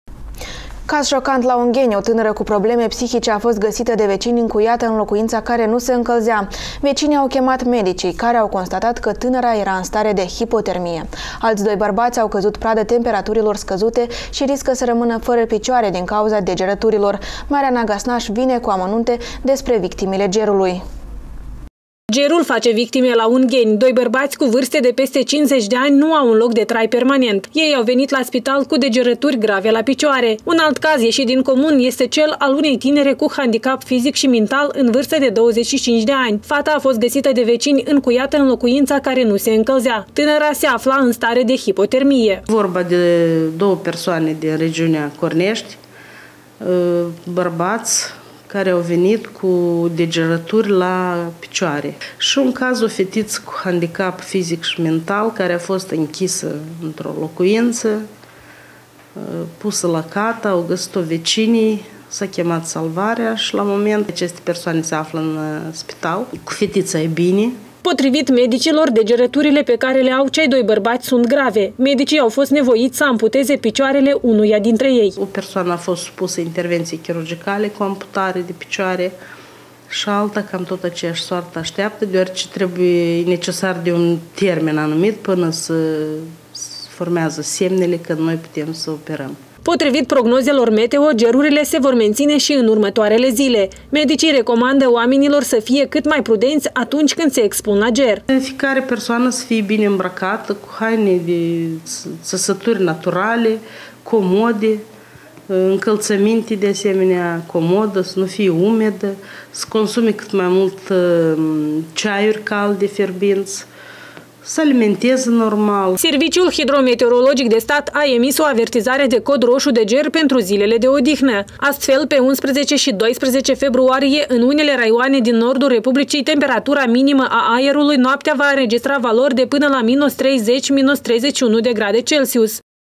O ungheneancă a fost găsită încuiată în casă care nu se încălzea
Caz șocant la Ungheni. O tânără cu probleme psihice a fost găsită de vecini încuiată în locuința care nu se încălzea. Vecinii au chemat medicii, care au constat că tânăra era în stare de hipotermie. Alți doi bărbați au căzut pradă temperaturilor scăzute şi riscă să rămână fără picioare din cauza degerăturilor.